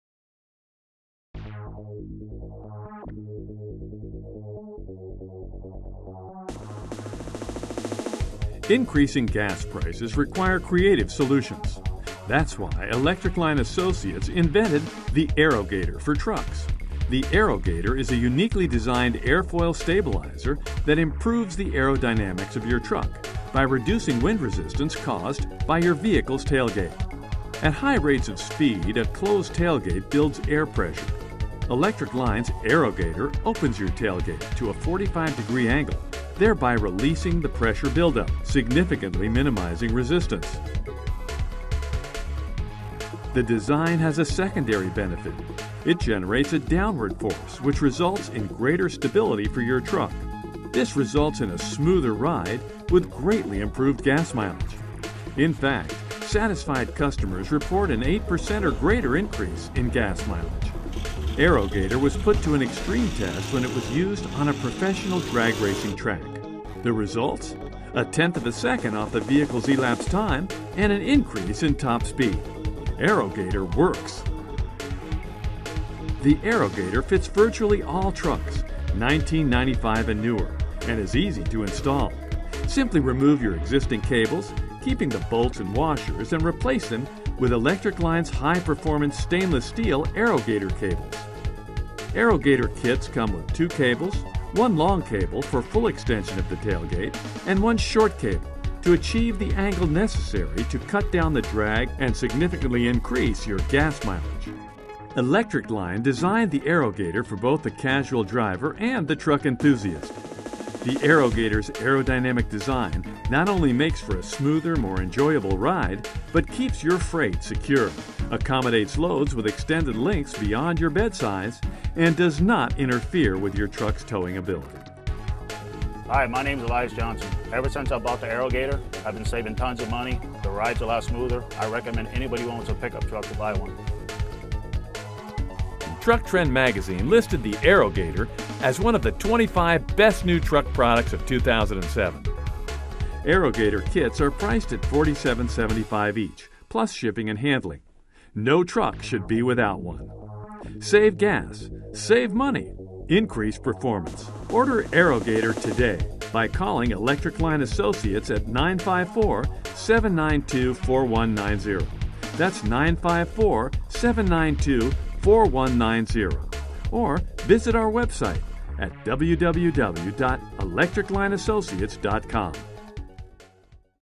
Product Promotional- Aerogator
Client came to us on a limited budget and asked that we create a promotional for his AeroGator product.  We delivered a product he was very happy with given his financial constraints.